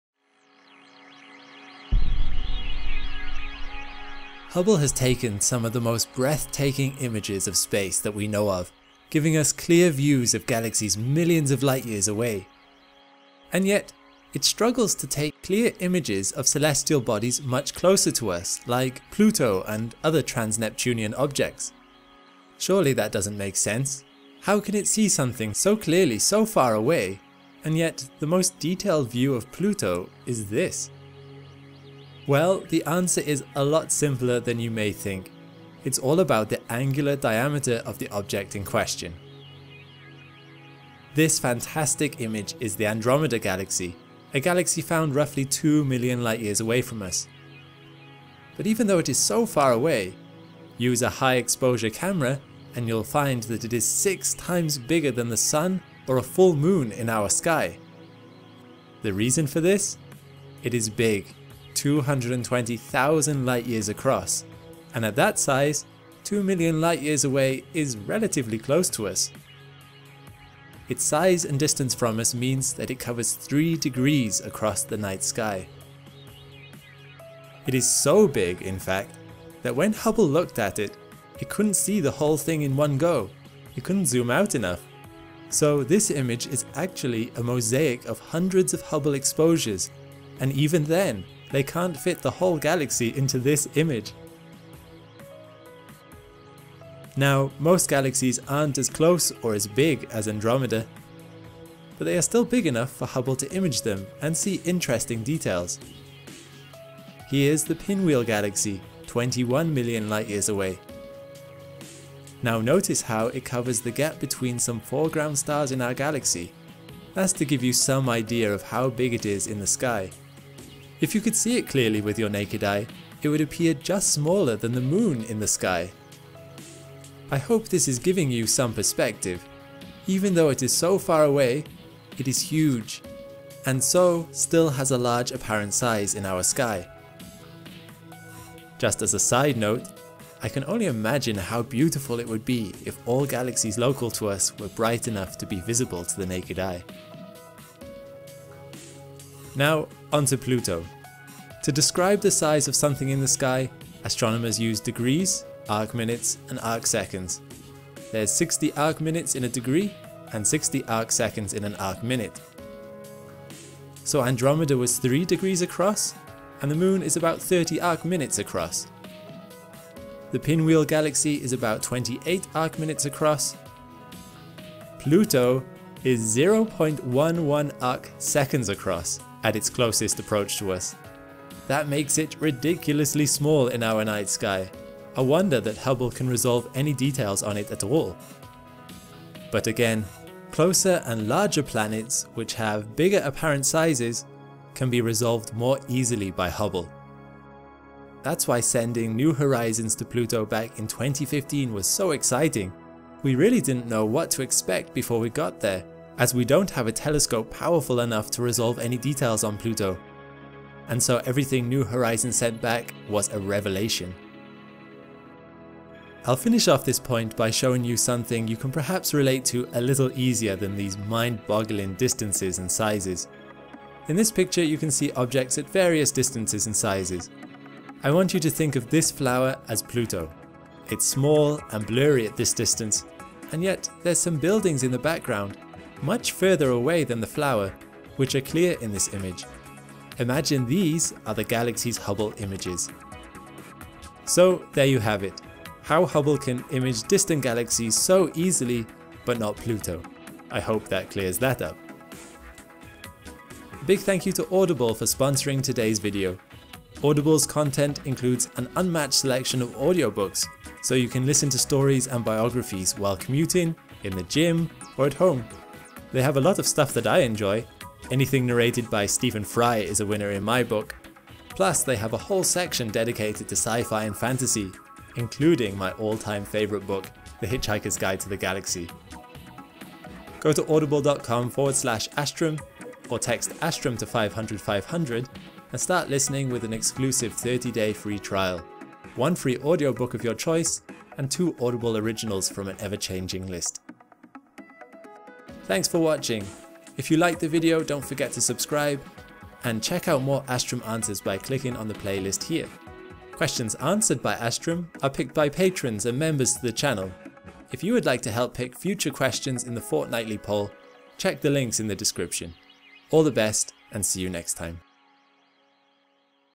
How Can Hubble See Distant Galaxies Clearly, But Not Pluto?
Hubble has taken some spectacular images of many different galaxies, but it can't resolve more than a few blobs on Pluto's surface. Why is that?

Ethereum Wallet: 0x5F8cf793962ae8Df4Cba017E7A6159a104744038

Image Credits: NASA